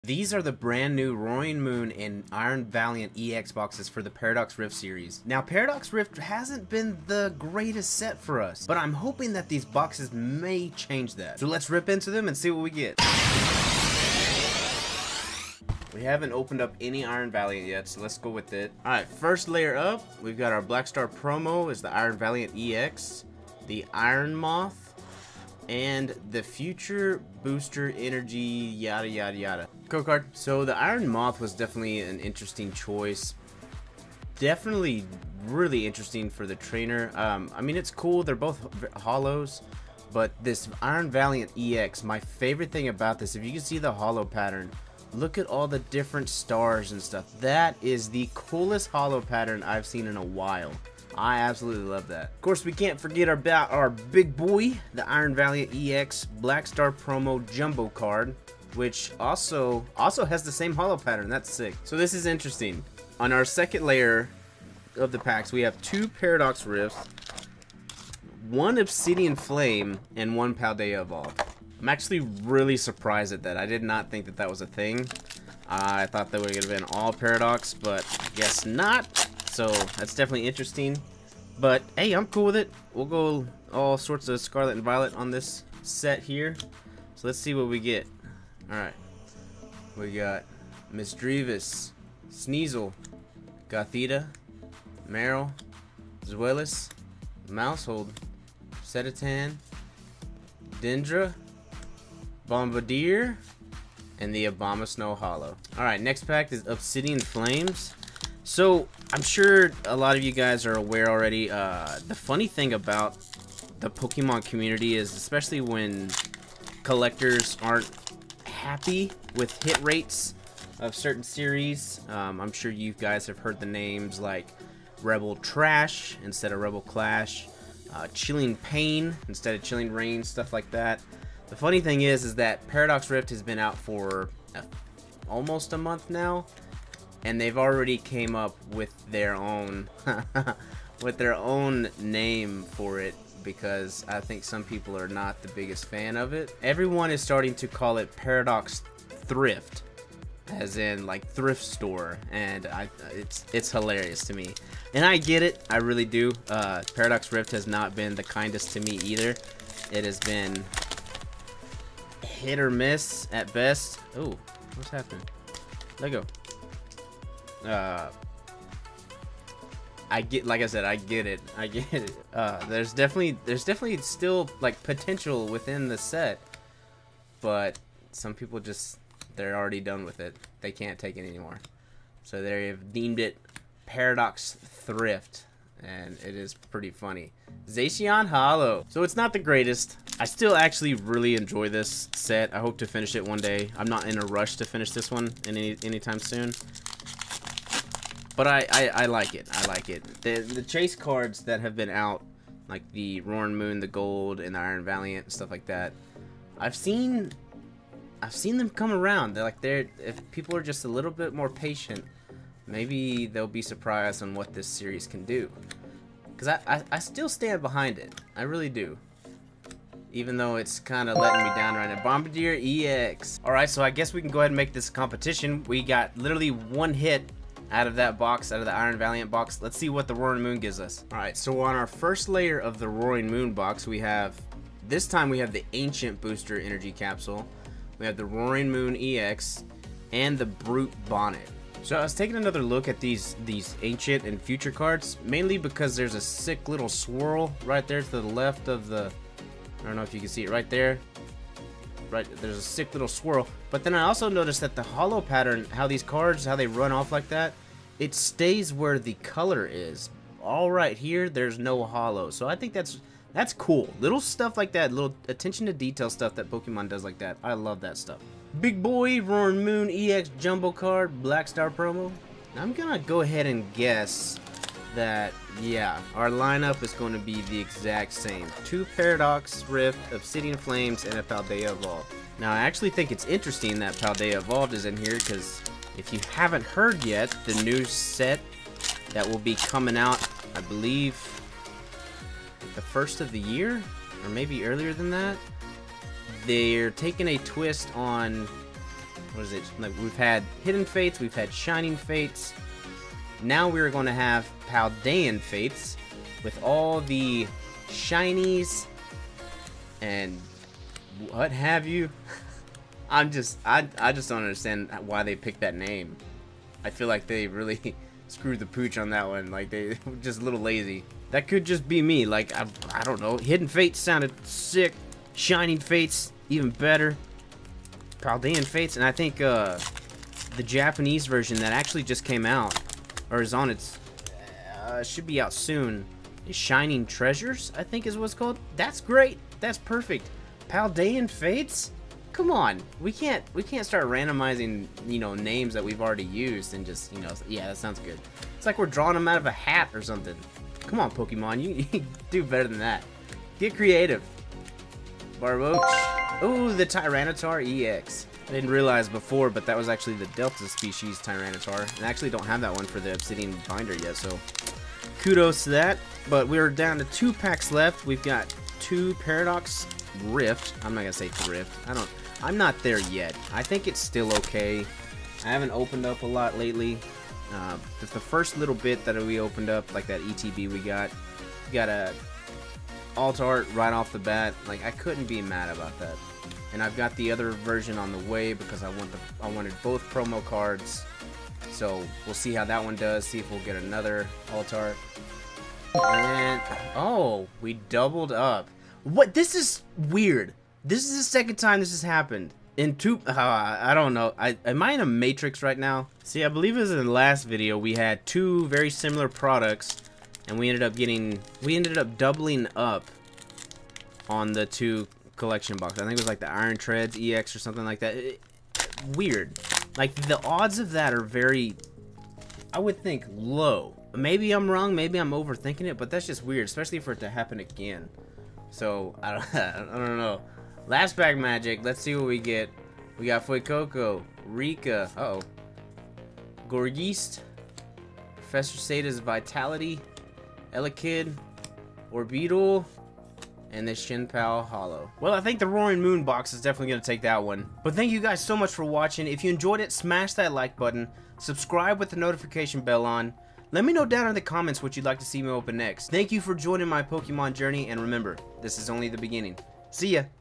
Is this Paradox Rift's LAST product?!?
In this opening we tackle the Roaring Moon EX box and the Iron Valiant EX box. Did Paradox Rift have too much hype behind it? Is the nickname "Paradox Thrift" fitting? We'll find out in this video!